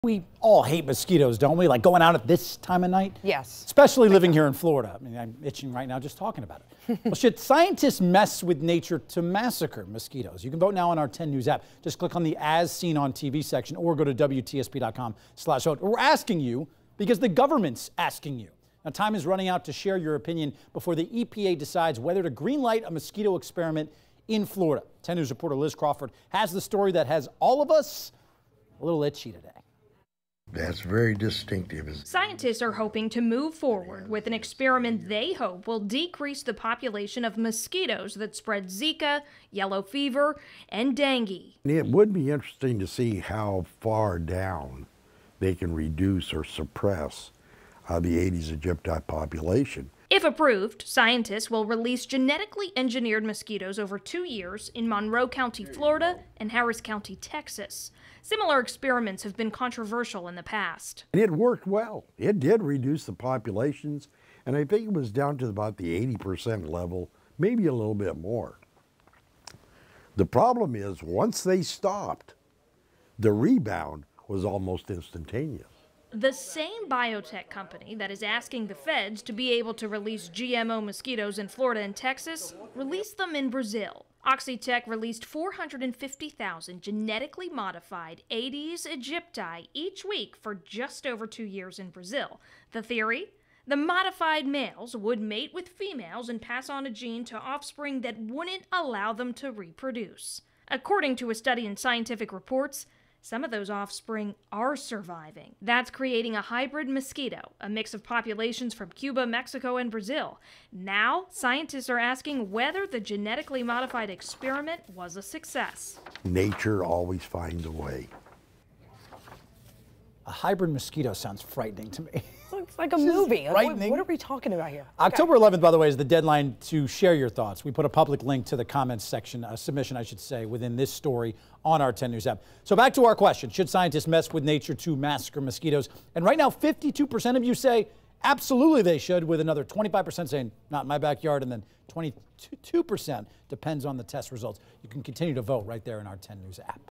Scientifically tricked-out mosquitoes sent to kill their own
The Environmental Protection Agency will decide whether to greenlight a mosquito experiment in Florida. The EPA is accepting public comment through Oct. 11.